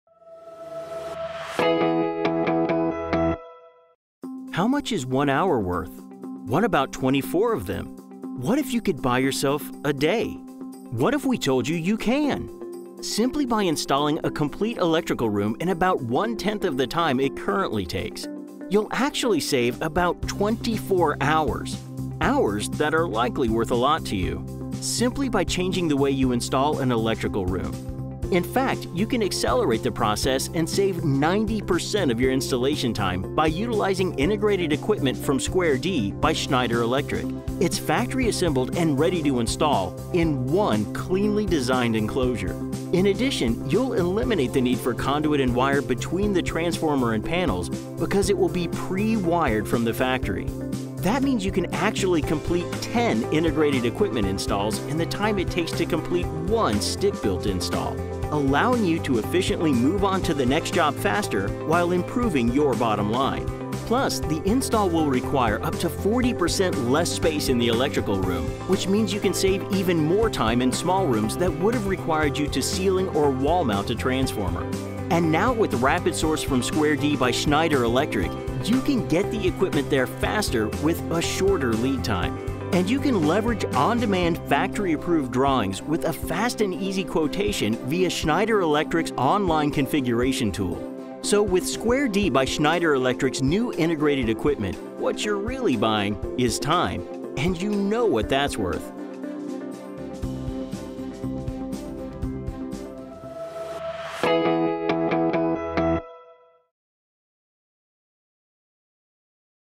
Space-Saving Unit: Integrated Equipment by Square D Cuts Labor Time | Schneider Electric
The prewired & factory assembled Integrated Equipment from Square D by Schneider Electric will save up to 90% labor time by combining panels & electrical transformers into one unit with a smaller footprint.

Order RapidSource™ Integrated Equipment and get it faster, with reduced lead time.

This video is about Square D's Integrated Equipment that will save installation time, saving your business time and money.